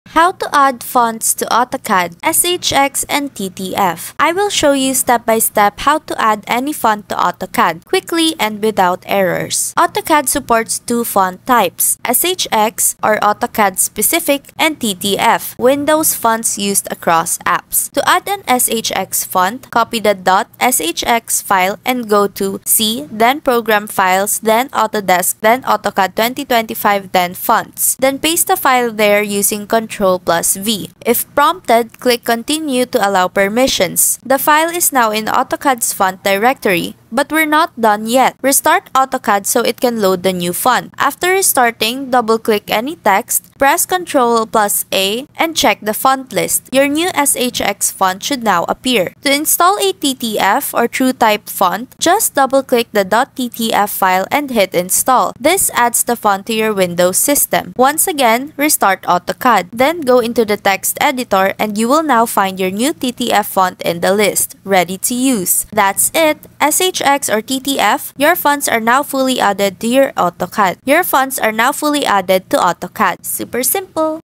How to Add Fonts to AutoCAD (SHX & TTF) – Easy Guide (2025)
Learn how to easily add fonts to AutoCAD in 2025! This step-by-step guide covers both SHX and TTF font types so you can customize your drawings with the perfect style. Whether you're a beginner or pro, this tutorial makes adding fonts to AutoCAD simple and quick.